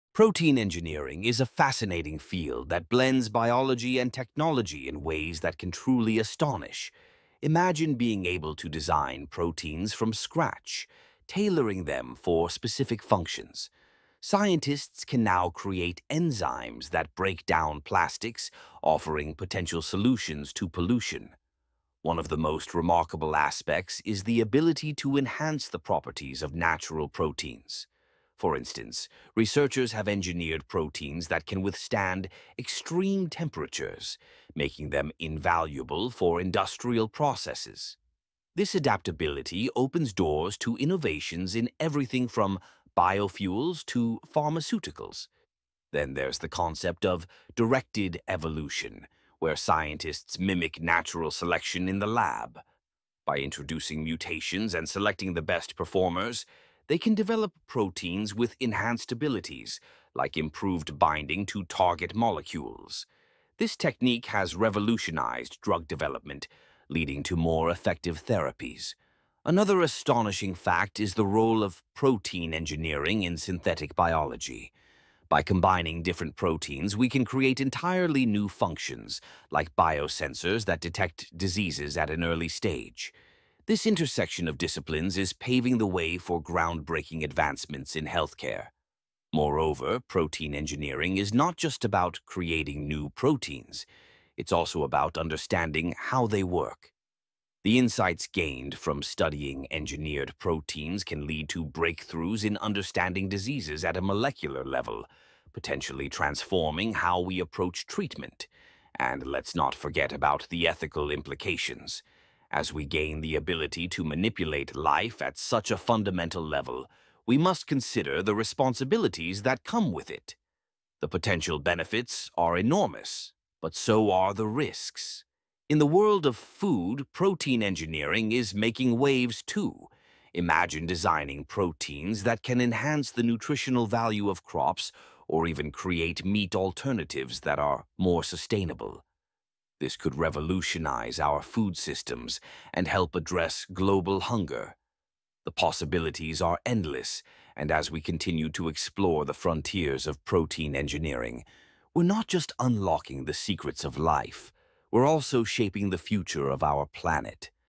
10 Astonishing Facts About Protein Engineering | KNOW iT
In this video, we dive into 10 astonishing facts about protein engineering—the cutting-edge field where biology meets design. From creating life-saving drugs and synthetic enzymes to building sustainable materials and even reprogramming organisms, protein engineering is unlocking the next frontier of biotechnology.

Whether you're fascinated by genetics, synthetic biology, or molecular design, this episode explores how scientists are reshaping nature—one protein at a time.

🧬 Because the code of life is now editable.